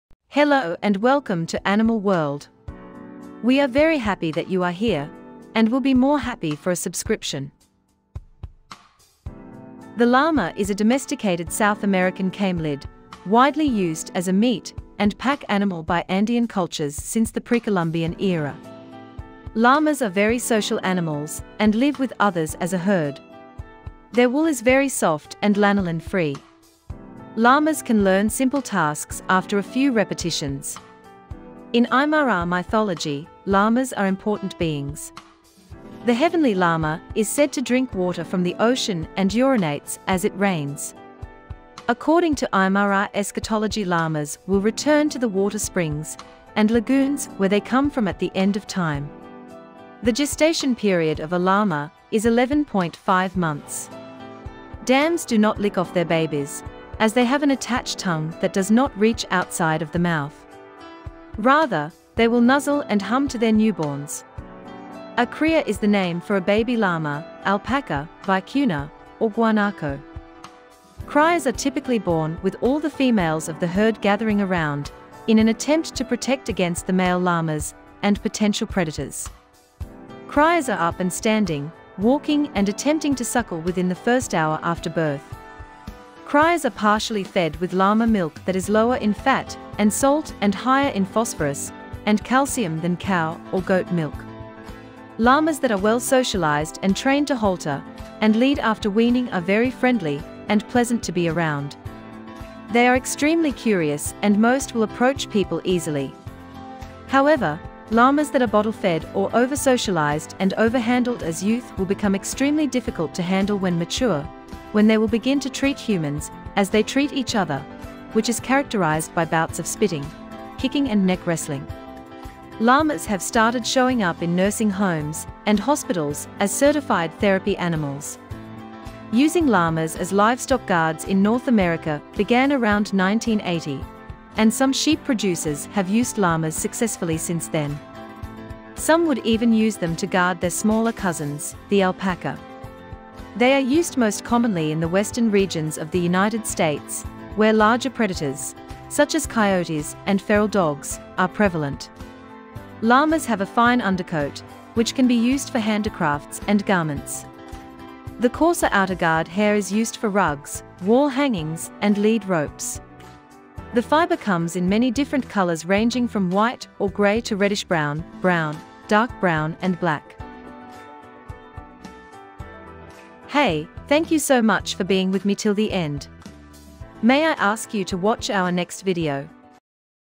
Llama facts 🐪 domesticated South American camelid
The llama is a domesticated South American camelid, widely used as a meat and pack animal by Andean cultures since the Pre-Columbian era.

Llamas are very social animals and live with others as a herd. Their wool is very soft and lanolin-free. Llamas can learn simple tasks after a few repetitions.
In Aymara mythology, llamas are important beings. The Heavenly Llama is said to drink water from the ocean and urinates as it rains. According to Aymara eschatology llamas will return to the water springs and lagoons where they come from at the end of time.
The gestation period of a llama is 11.5 months. Dams do not lick off their babies, as they have an attached tongue that does not reach outside of the mouth. Rather, they will nuzzle and hum to their newborns.
A cria is the name for a baby llama, alpaca, vicuña, or guanaco. Crias are typically born with all the females of the herd gathering around, in an attempt to protect against the male llamas and potential predators. Crias are up and standing, walking and attempting to suckle within the first hour after birth. Crias are partially fed with llama milk that is lower in fat and salt and higher in phosphorus and calcium than cow or goat milk.
Llamas that are well-socialized and trained to halter and lead after weaning are very friendly and pleasant to be around. They are extremely curious and most will approach people easily. However, llamas that are bottle-fed or over-socialized and over-handled as youth will become extremely difficult to handle when mature, when they will begin to treat humans as they treat each other, which is characterized by bouts of spitting, kicking and neck wrestling.
Llamas have started showing up in nursing homes and hospitals as certified therapy animals.
Using llamas as livestock guards in North America began around 1980, and some sheep producers have used llamas successfully since then. Some would even use them to guard their smaller cousins, the alpaca. They are used most commonly in the western regions of the United States, where larger predators, such as coyotes and feral dogs, are prevalent.
Llamas have a fine undercoat, which can be used for handicrafts and garments. The coarser outer guard hair is used for rugs, wall-hangings and lead ropes. The fiber comes in many different colors ranging from white or grey to reddish-brown, brown, dark brown and black.